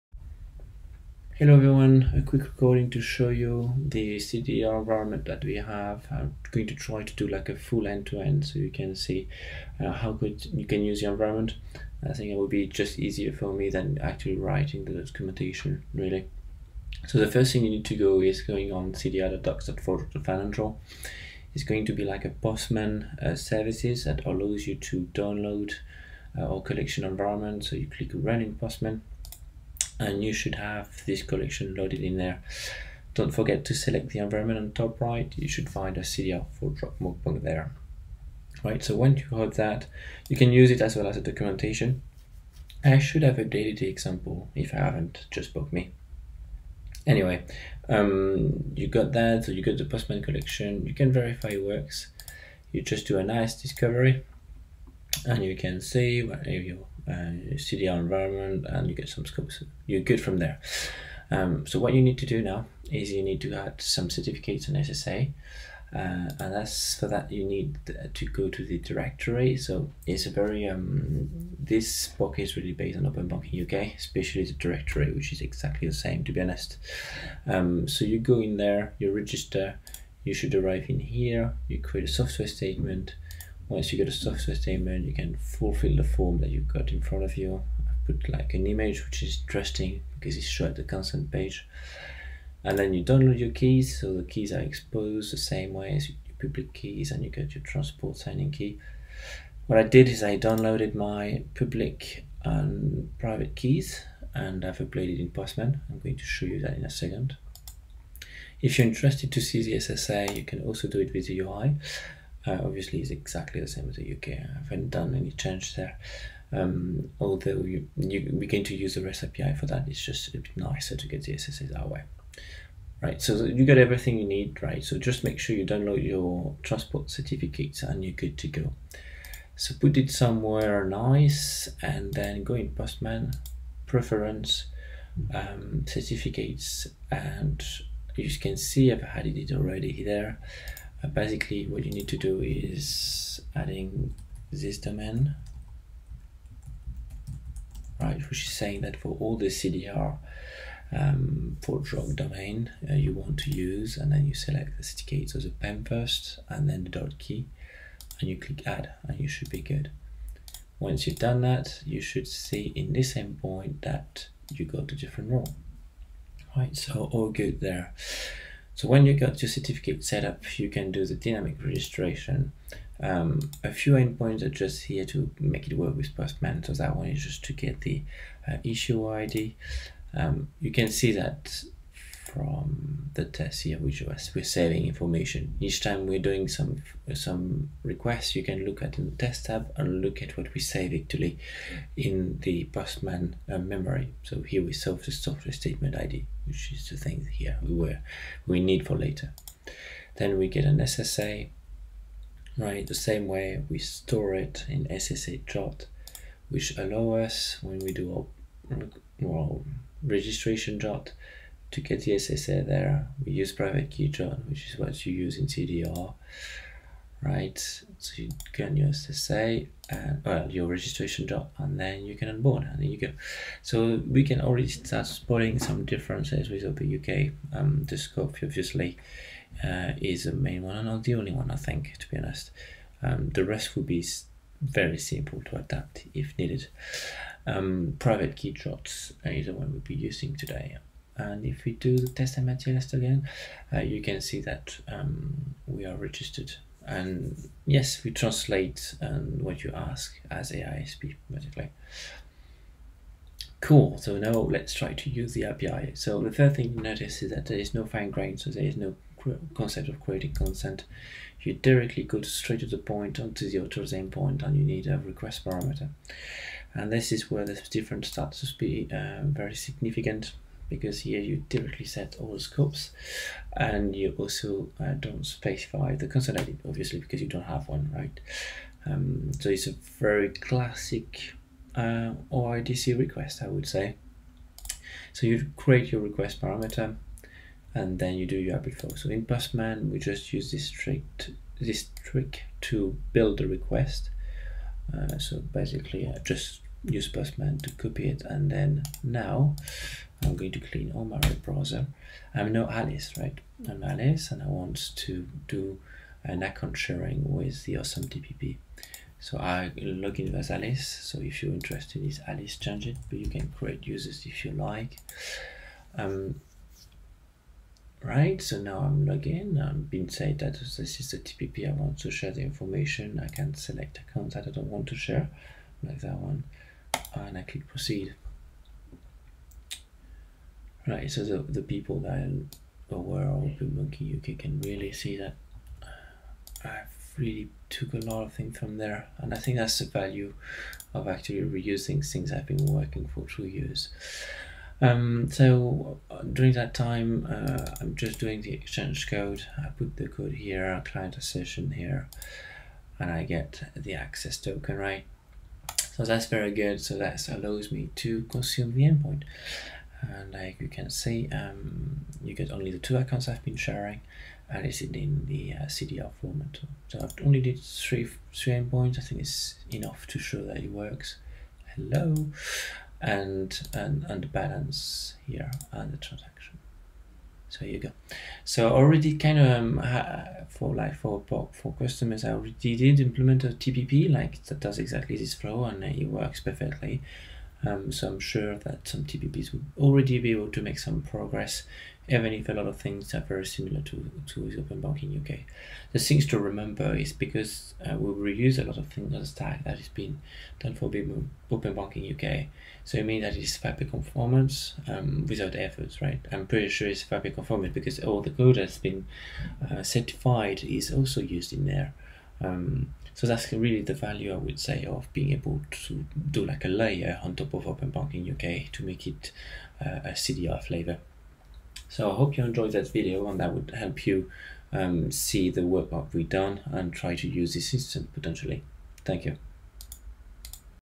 ForgeRock CDR Demo
A quick recording to demonstrate the CDR POC environment made publicly available by ForgeRock